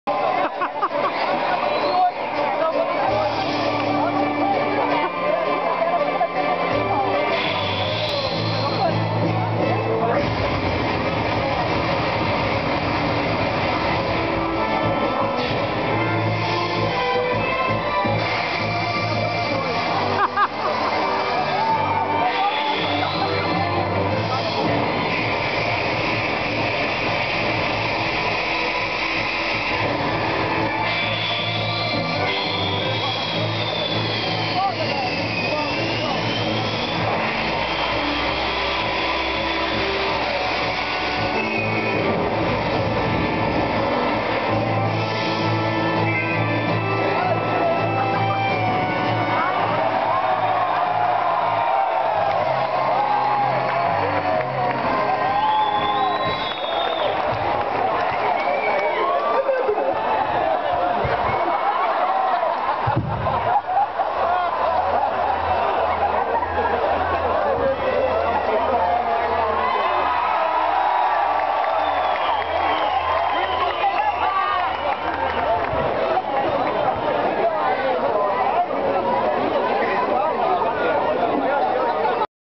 Jumbo Cactuar AnimeCon 2008(São Paulo 27.07.08)
Cosplayer do chefe de Final Fantasy 8 enfrentando um Squall imaginario...